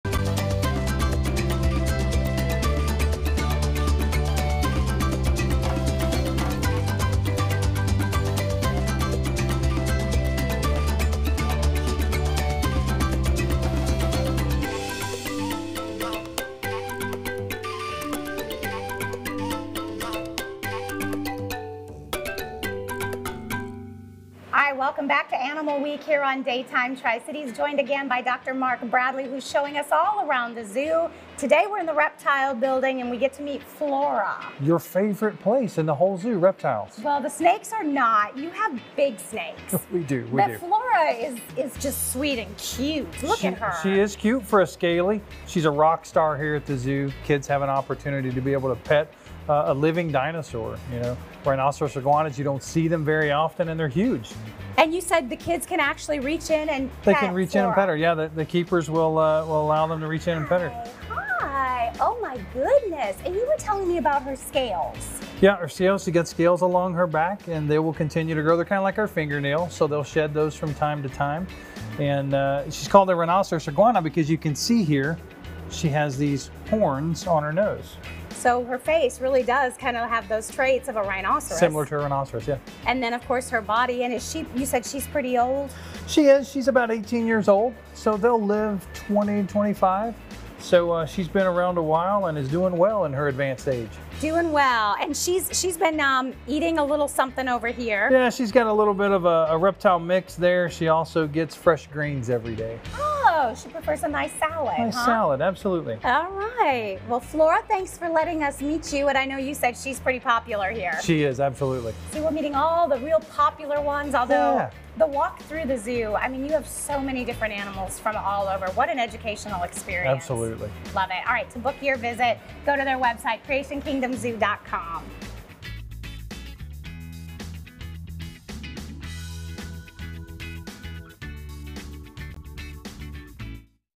Daytime Animal Week: Iguana
Amy takes us to Creation Kingdom Zoo for today's segment of Animal Week , as we take a look at an amazing Iguana!